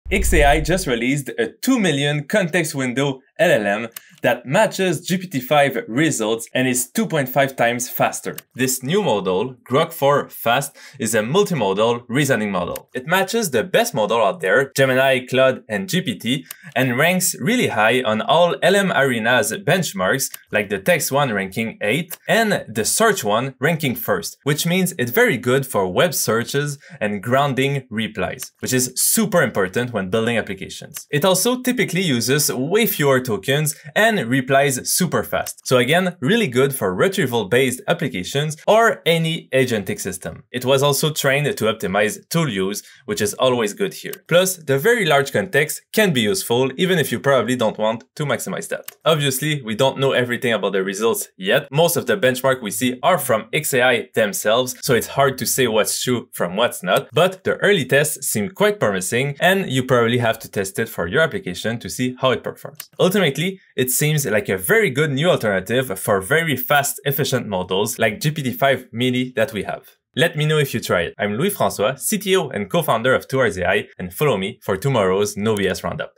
2M Context. 2.5x Faster. Is Grok 4 Fast Real?
Grok 4 Fast just dropped with a bonkers 2,000,000-token context and claims of up to 2.5x GPT-5 API speed while matching results.

It’s multimodal, trained end to end for tool use, and reportedly uses 40 to 50% fewer thinking tokens than peers.

xAI says it tops LMArena Search Arena and performs strongly on text, plus pricing is wild: $0.20 per 1M input, $0.50 per 1M output, which they frame as 23 to 25x cheaper than comparable setups and on average 47x cheaper than Grok 4 due to shorter reasoning.

Caveat: these are xAI’s numbers.